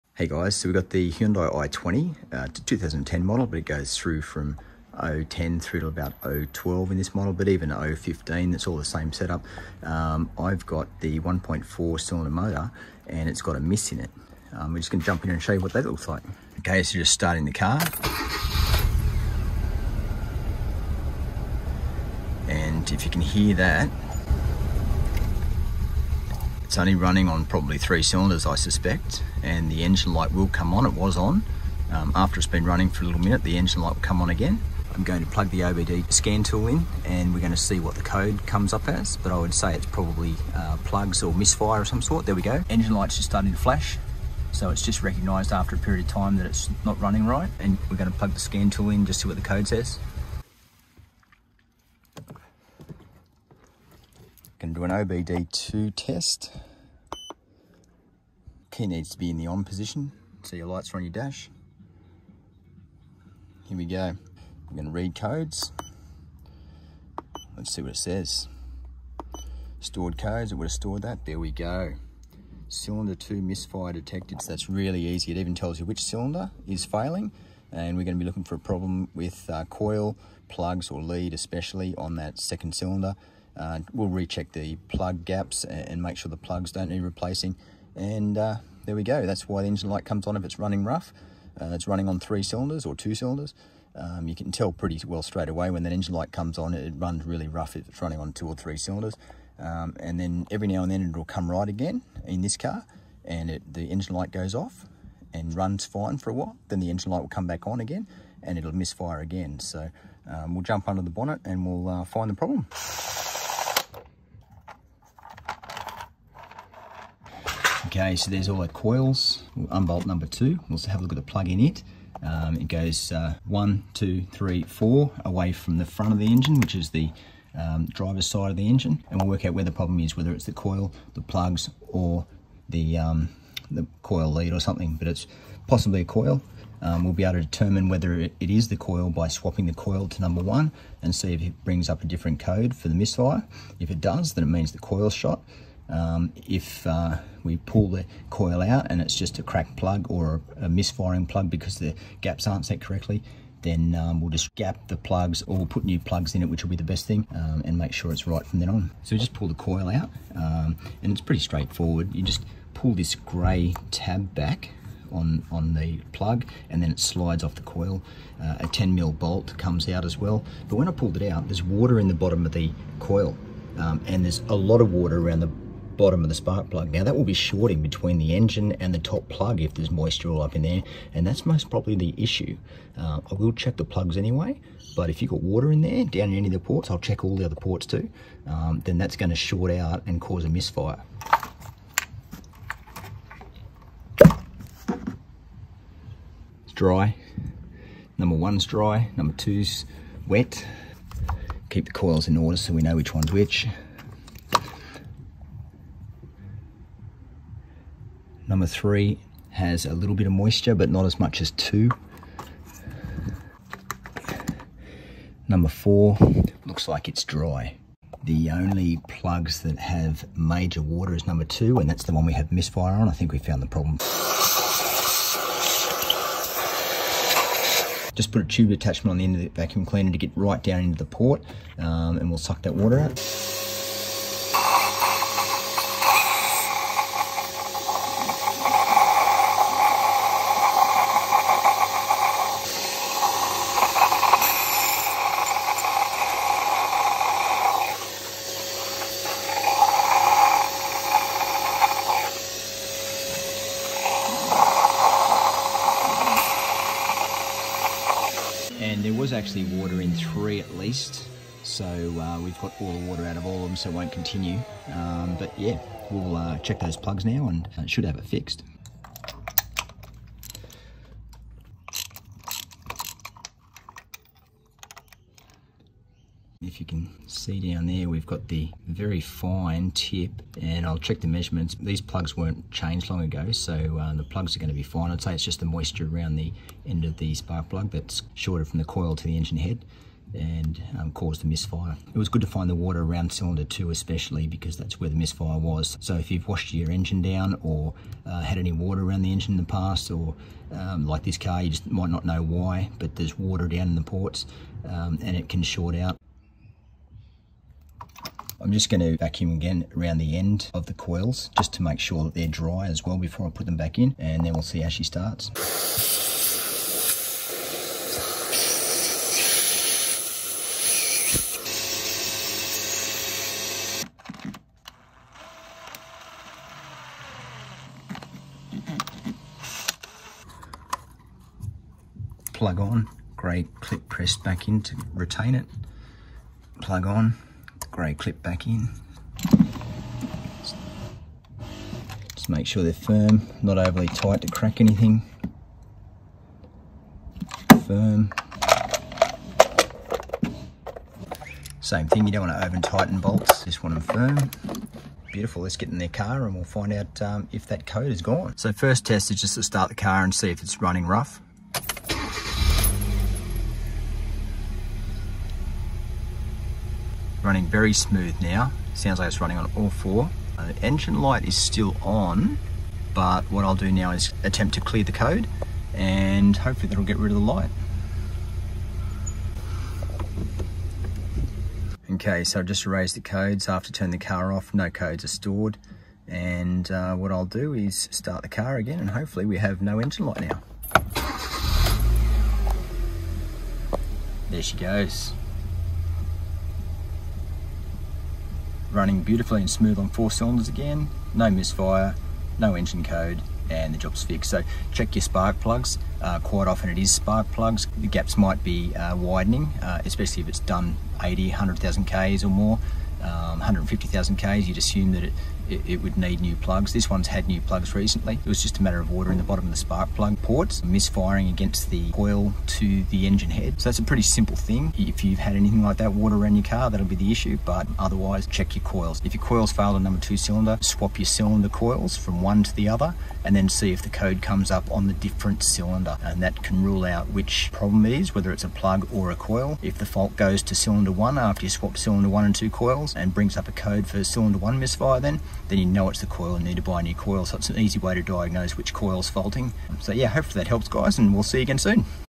Engine misfire running rough coils plugs & leads Hyundai I20 also most 4 6 & 8 cylinder Toyota Mazda
When an engine drops a cylinder, it can be a few possible causes, but it will sound more irregular as it runs rough rather than smoothly on all cylinders.
This video will hopefully help you with the procedure to tracking down the causes & trouble shooting which exact part is the cause of your misfire or rough running engine.